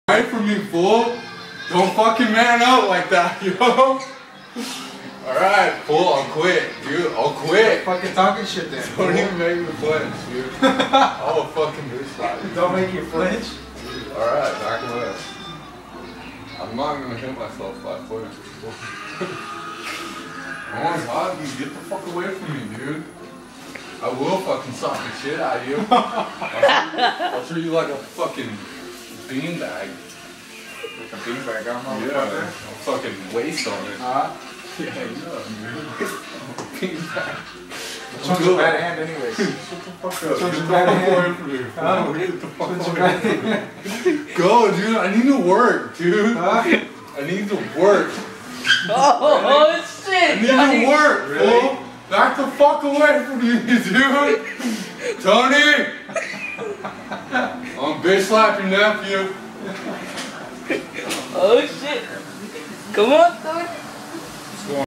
Juan and chad arguing
Juan and chad at the barbershop